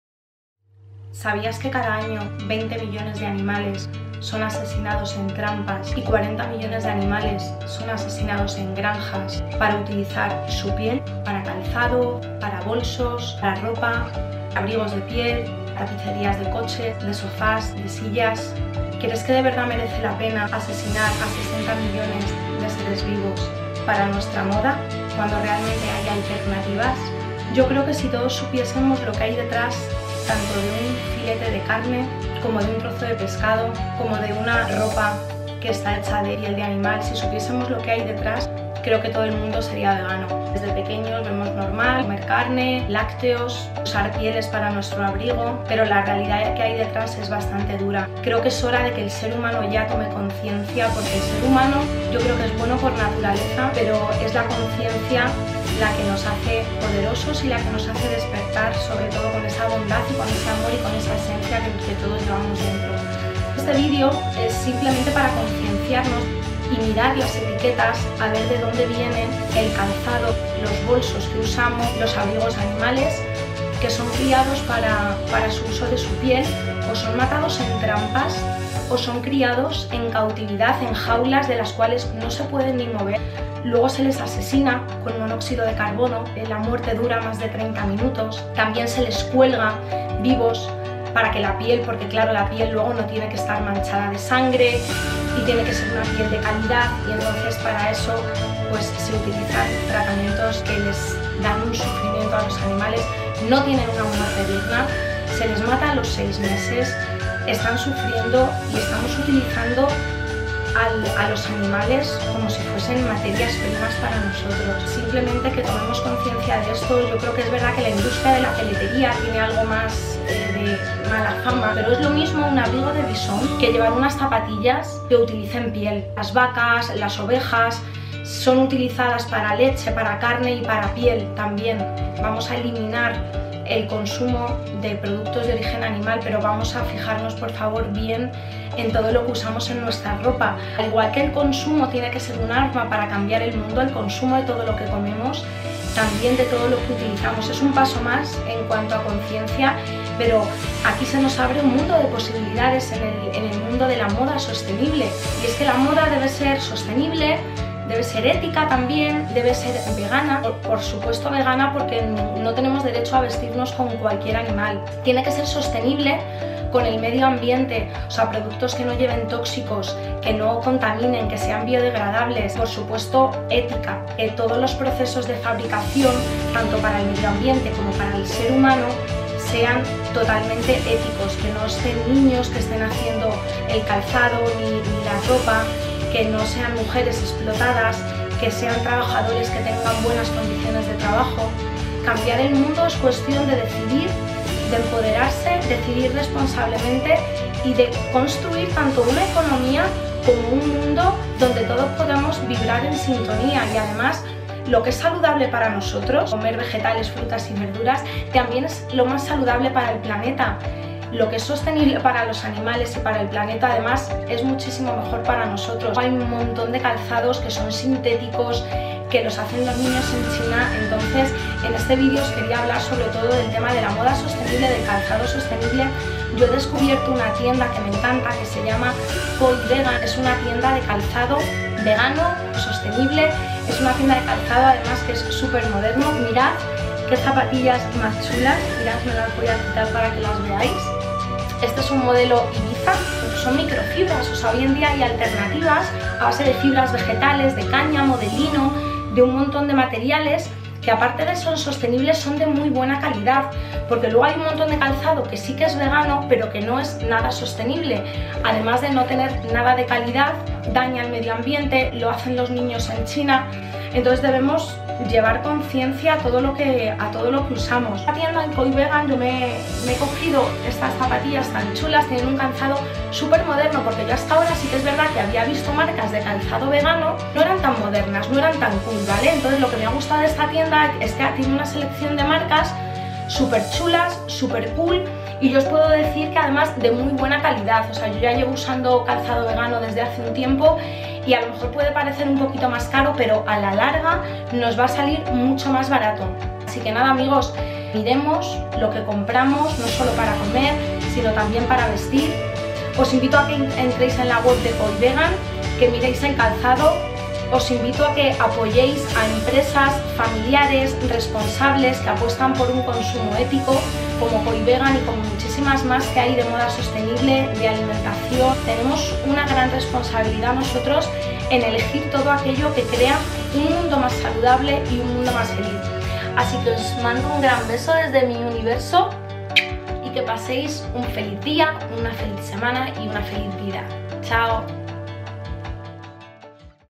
POR QUÉ USAR MODA VEGANA
Para estar a la moda no hay falta usar piel. Los animales no son ingredientes ni materias primas para nuestro uso o disfrute. Por tu salud, por la del planeta, por los animales, y por construir una nueva economía verde, sostenible y vegana. Mis zapatillas las podéis encontrar en koivegan: